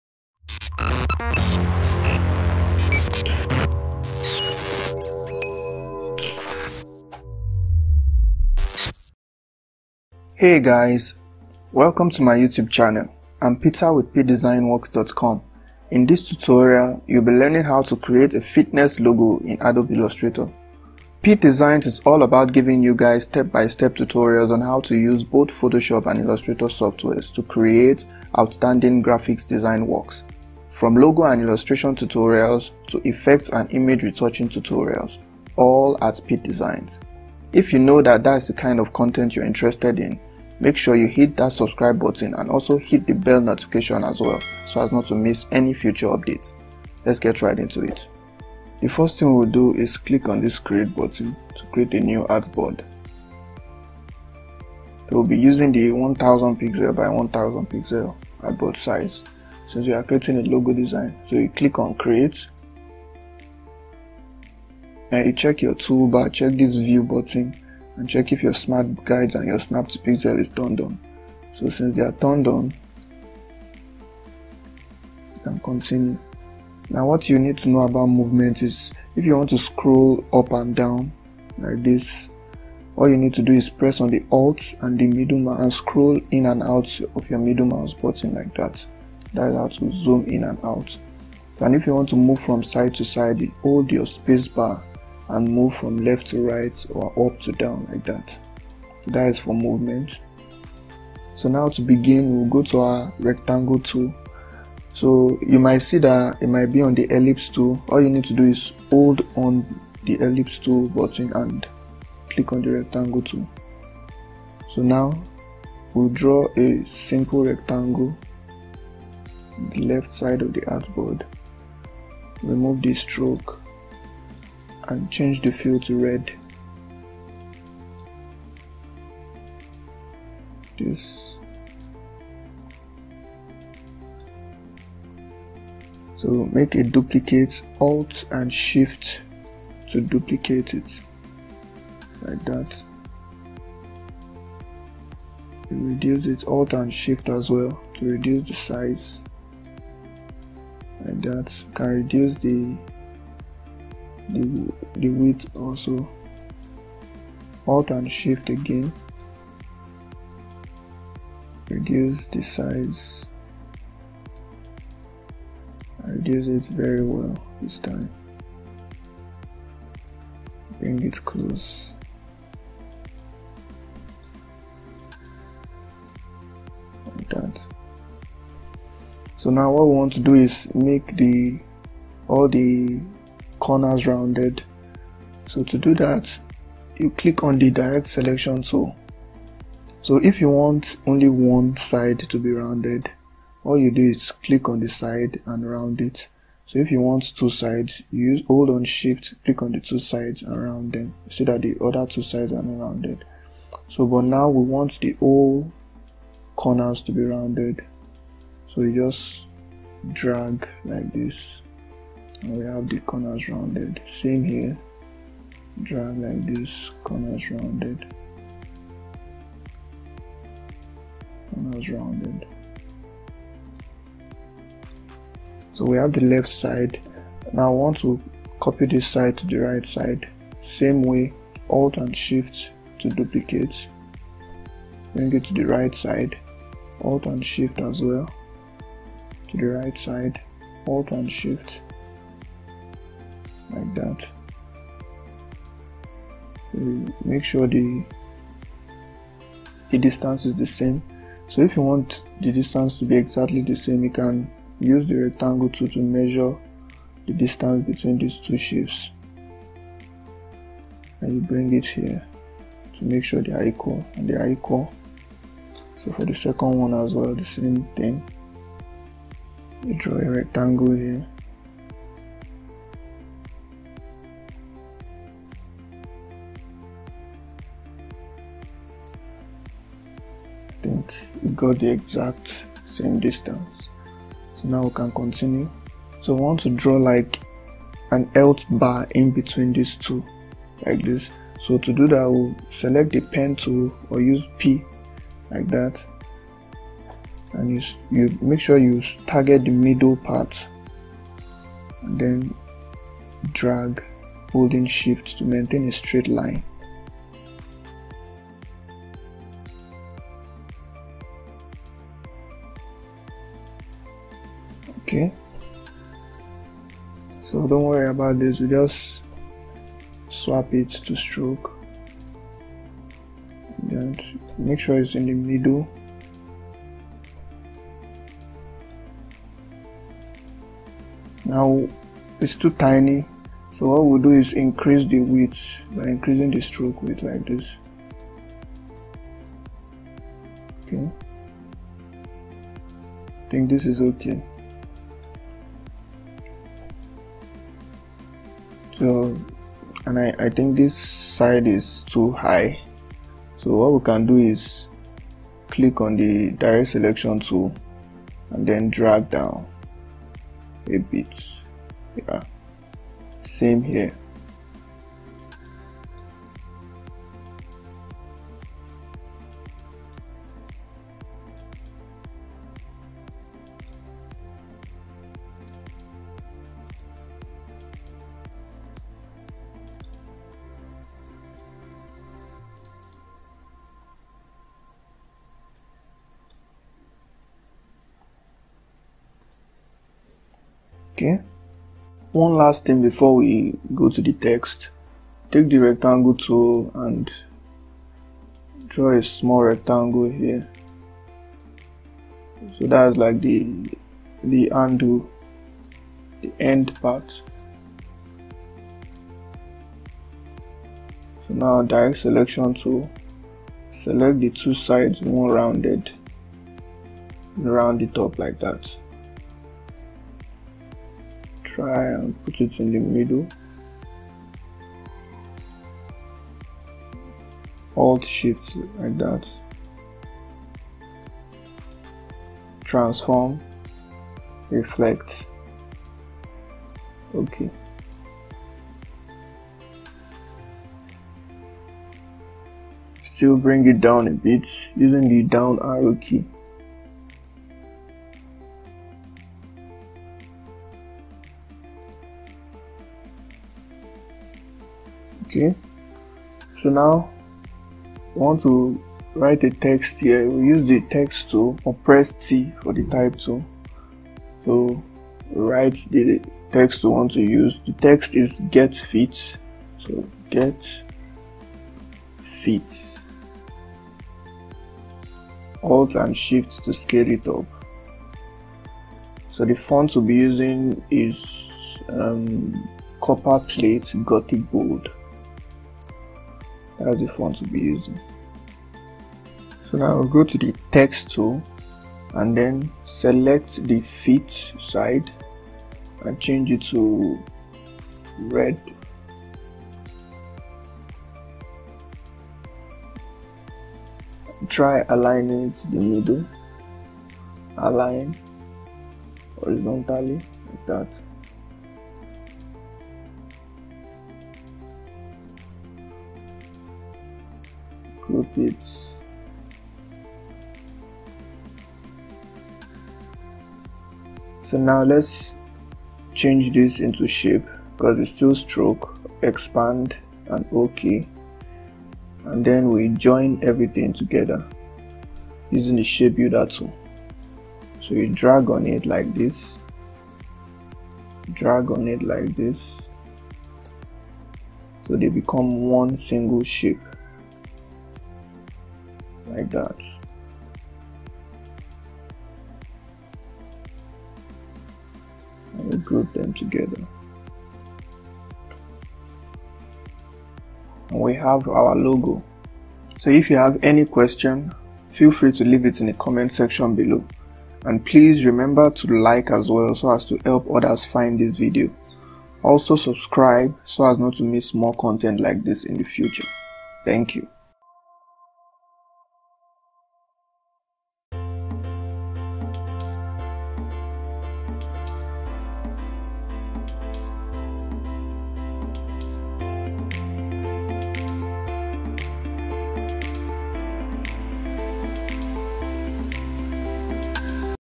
How to create a fitness logo | Adobe illustrator beginner tutorial
In this beginner-friendly tutorial, I'll be showing you how to create a fitness logo using adobe illustrator.